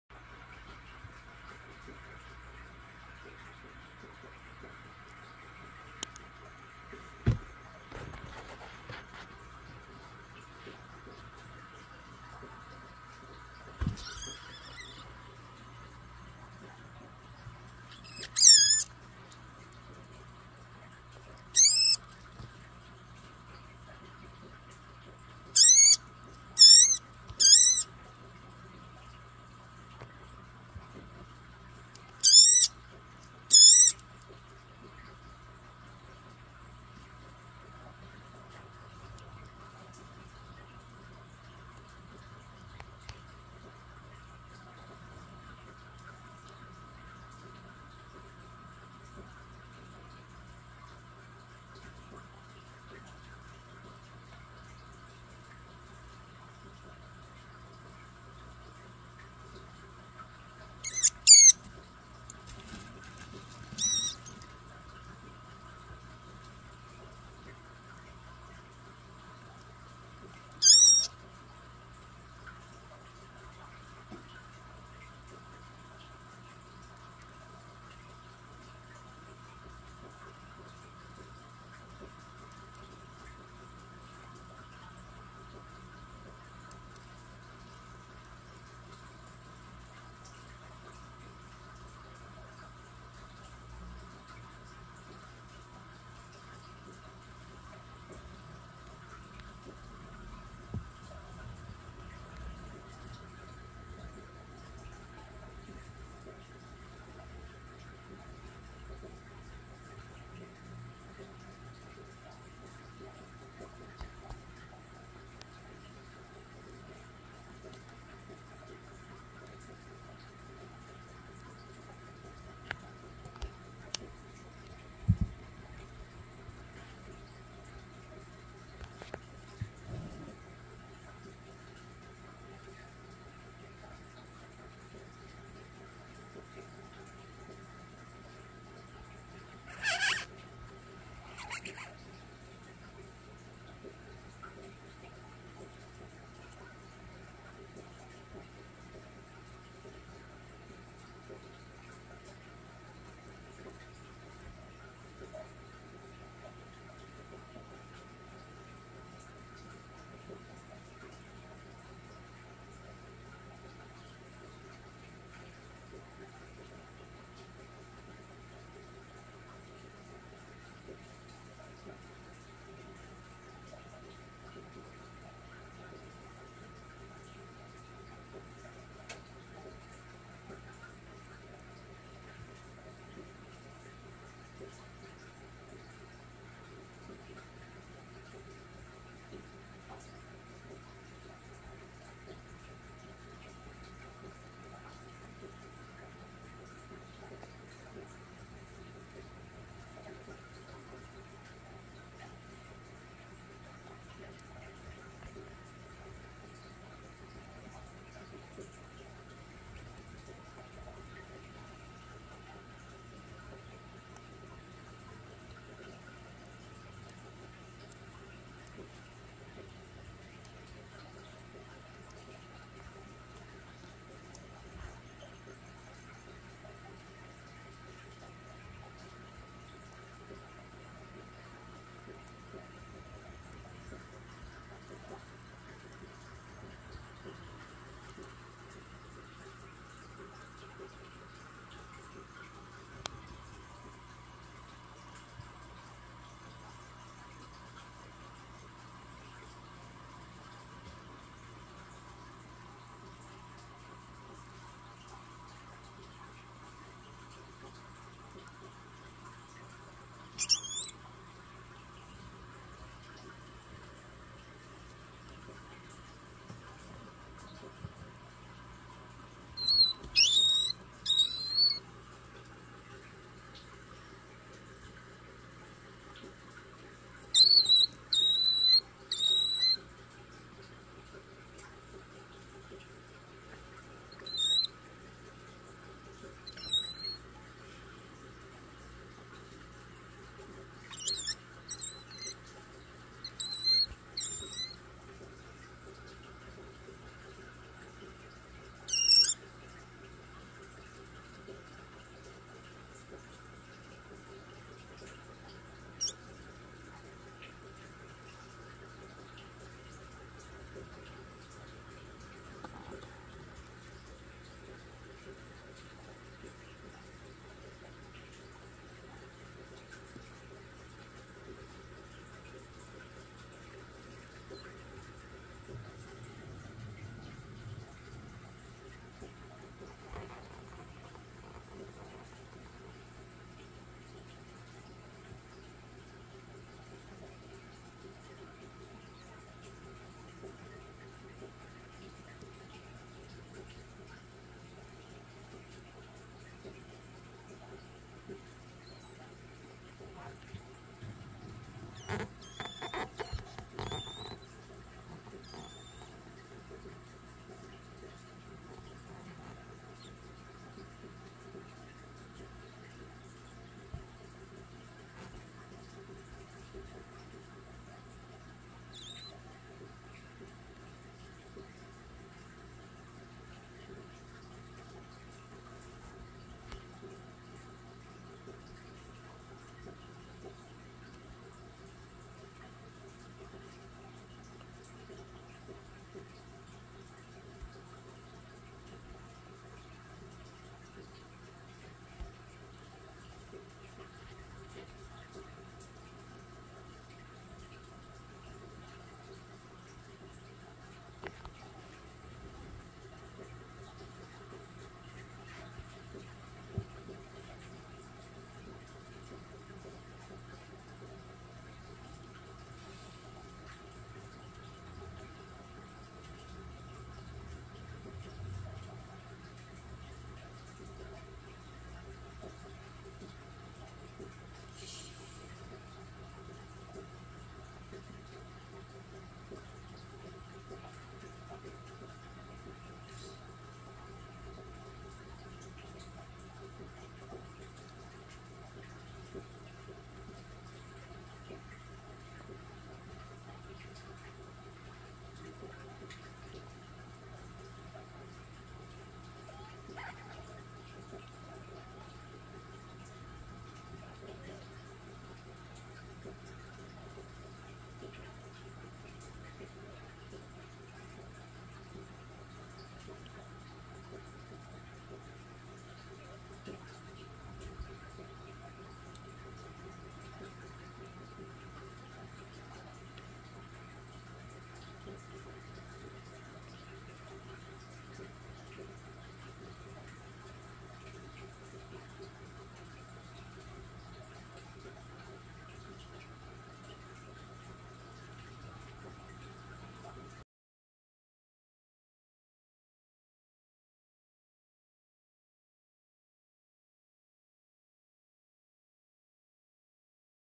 Python Eats Live Rat / Warning Live Feeding Documentation
in this video we document a ball python eating a rat .